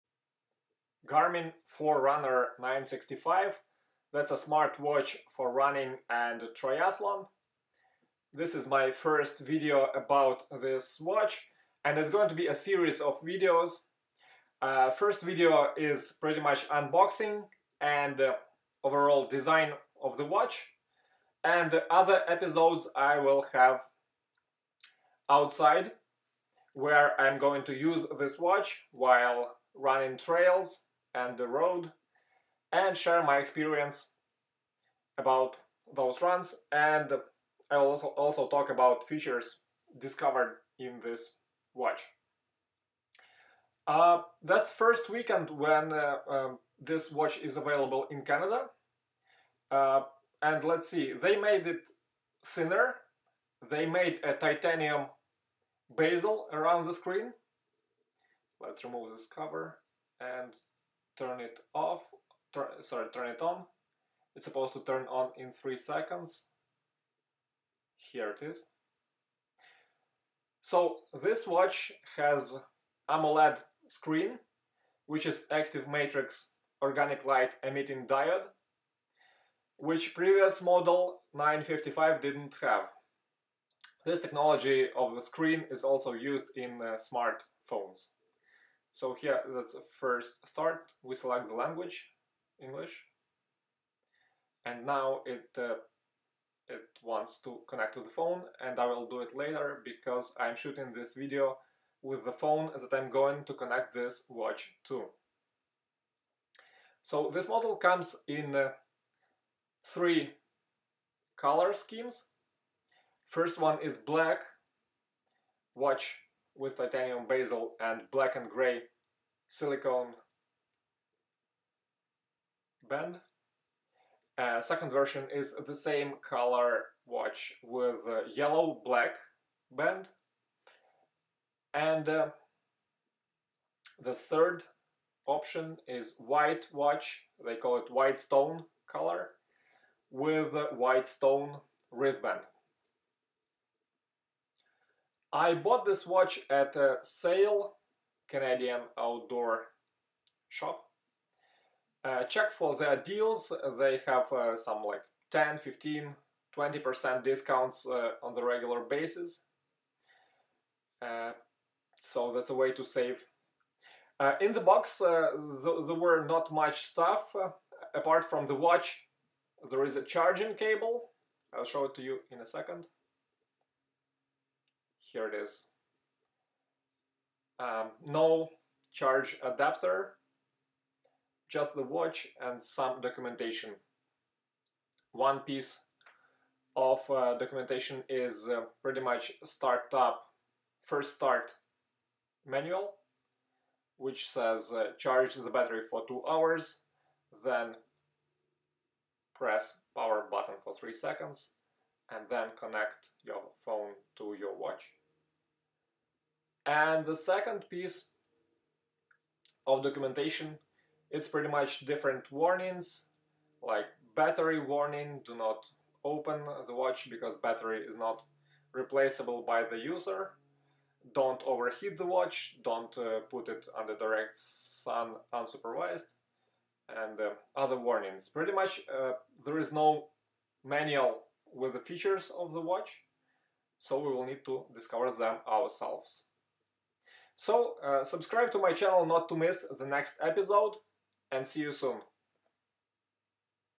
Garmin Forerunner 965 Review EP1: Unboxing & Design
Just got this new GPS smartwatch for Running and Triathlon a few hours ago. It is the first weekend it is available in Canada. This is the first episode of my review and the only one made indoors. All the rest will be in the field using the watch, sharing feedback and reviewing the features.